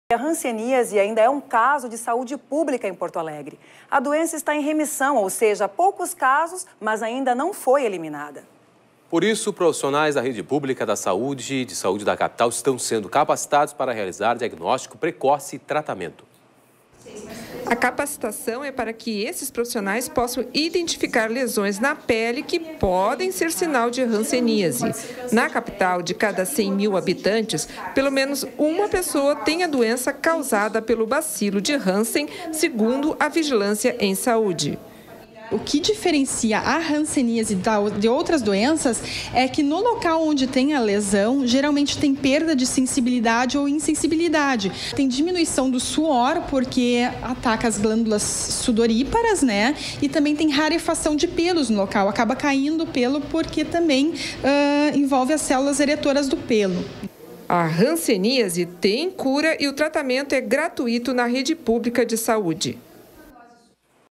Profissionais da saúde são capacitados para diagnóstico precoce da hanceníase
A hanseníase ainda é um caso de saúde pública em Porto A|legre.  A doença está em remissão, ou seja, há poucos casos mas ainda não foi eliminada. Por isso, profissionais da rede básica de saúde na capital estão sendo capacitados para realizar diagnóstico precoce e tratamento.